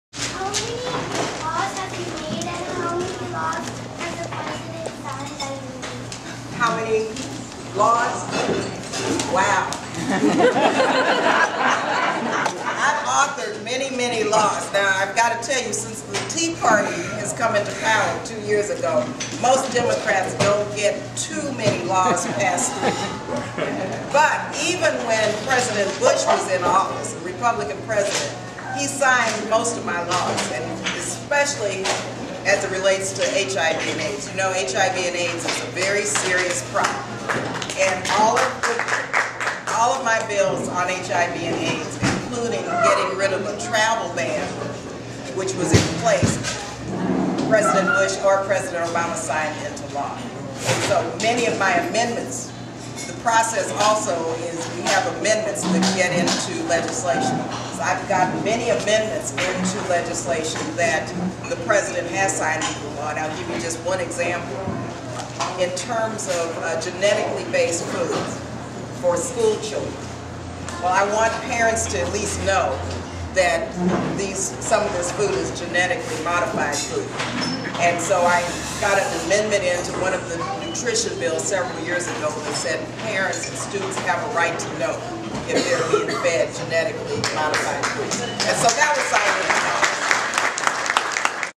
U.S. Representative Barbara Lee on How Many of Her Bills Obama Has Signed
U.S. Representative Barbara Lee answers a question on how many of her bills have been signed into law by President Obama.